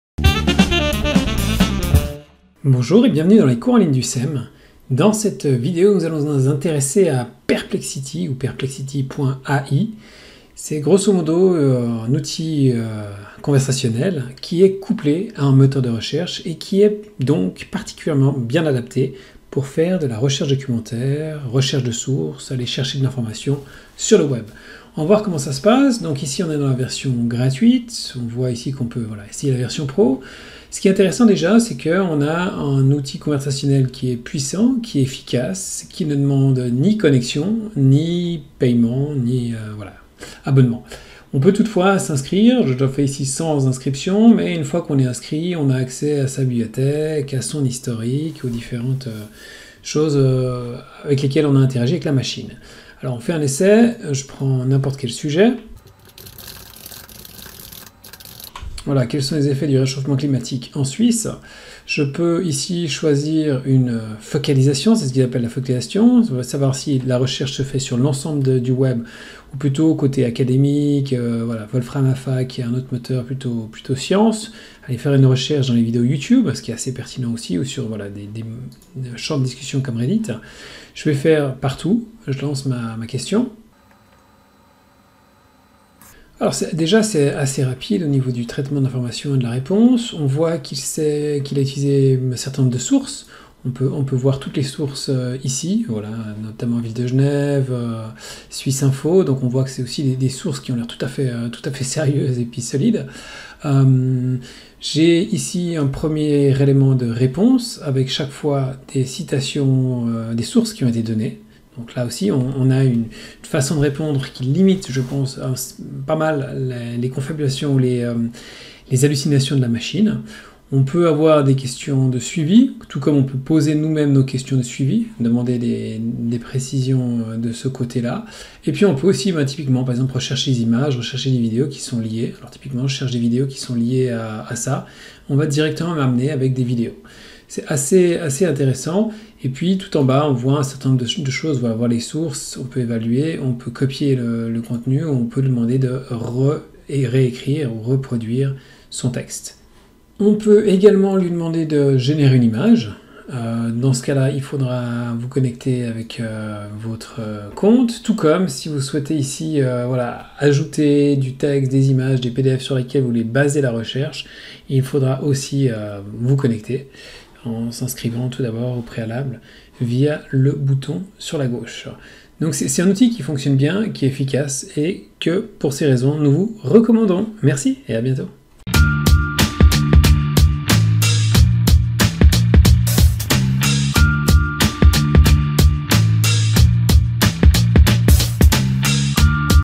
Perplexity.ai
Dans cette brève vidéo, nous vous expliquons comment utiliser Perplexity.ai, et ce notamment pour faire de la recherche documentaire.